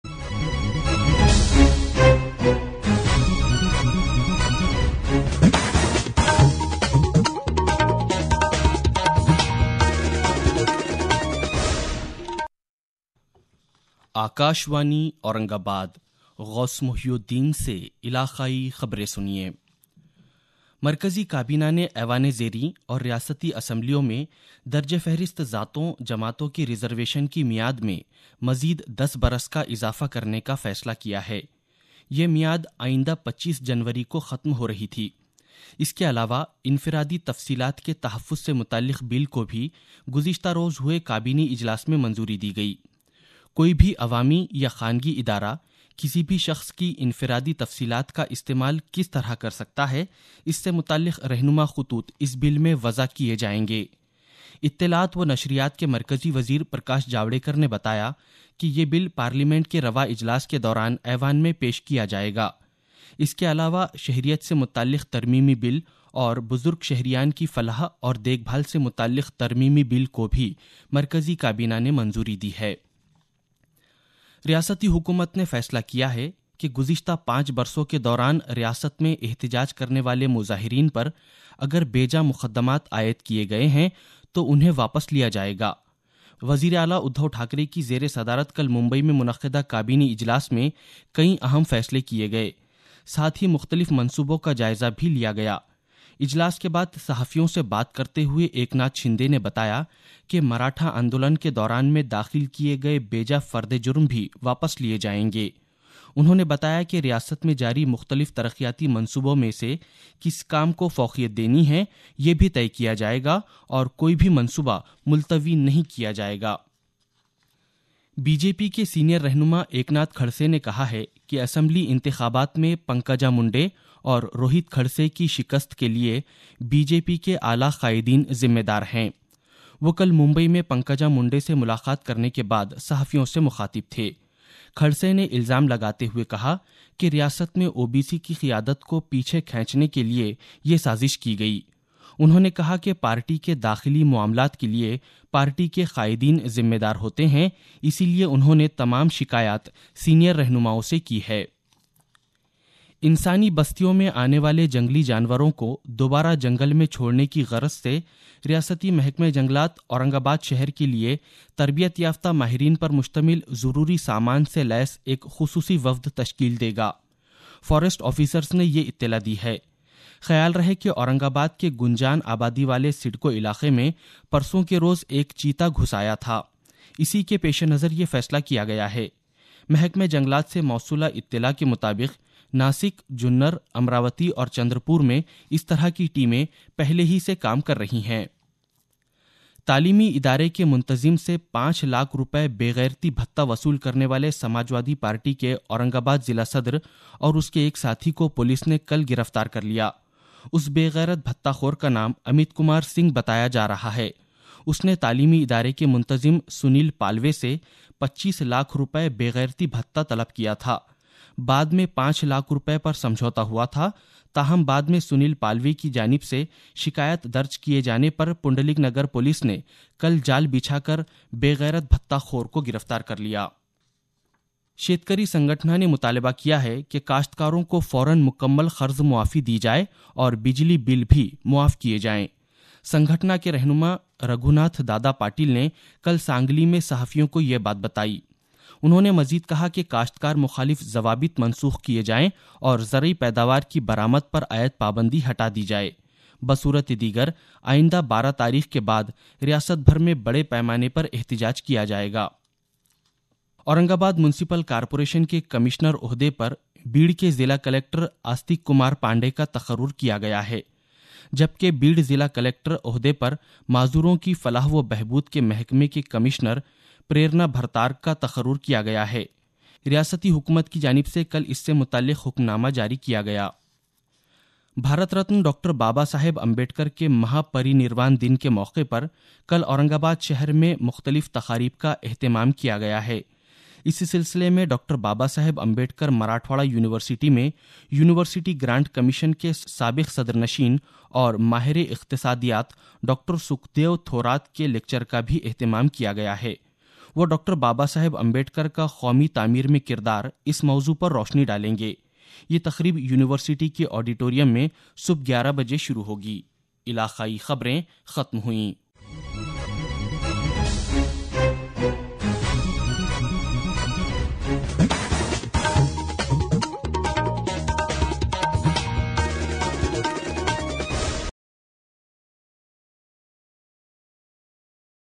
AIR News Urdu Bulletin, Aurangabad. Date : 05.12.2019, Time : 8.40 - 8.45 AM
انسانی بستیوں میں آنے والے جنگلی جانوروں کو دو بارہ جنگل میں چھو ڑ نے کی غرض سے ریاستی محکمہ جنگلات  اورنگ آ باد شہر کے لیے تر بیت یافتہ ماہرین پر مشتمل ضروری سامان سے لیس ایک خصو صی وفد تشکیل دے گا۔